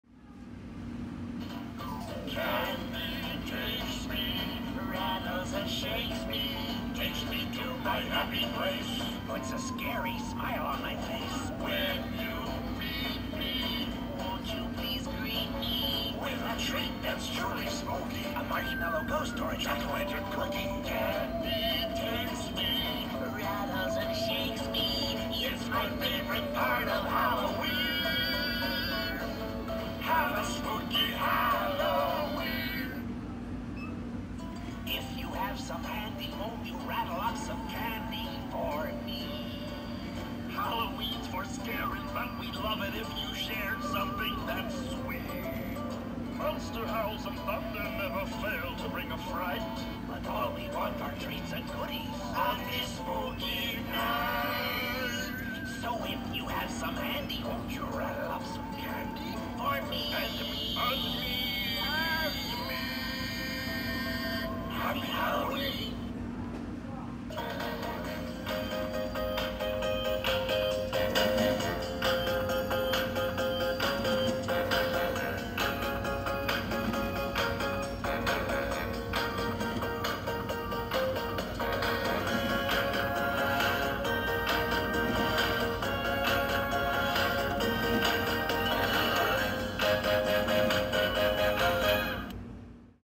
Bat and Skull Radio | Halloween 2020
Sold at Big Lots for $30.00. Made by Gemmy Industries Corp.

Recorded at:
Big Lots